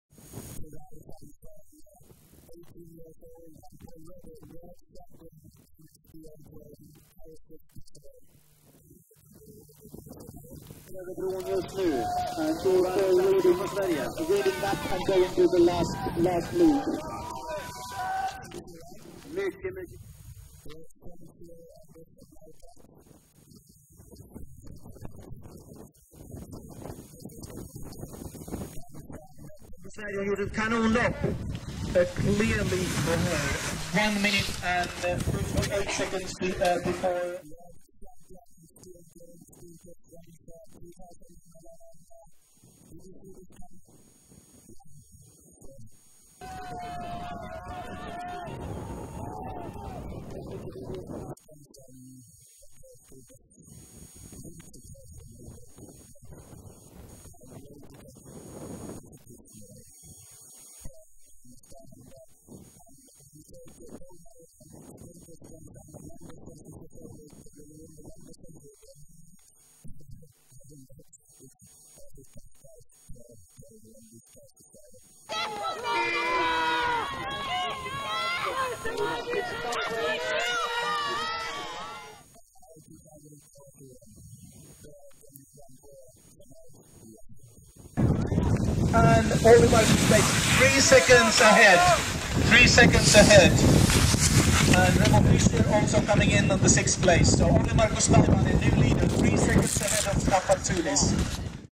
Ski WOC 2011: Sprint winners
Tove Alexandersson and Olli-Markus Taivainen became the first world champions in Ski Orienteering 2011. Sprint distance from Tänndalen in Sweden 22/3 2011.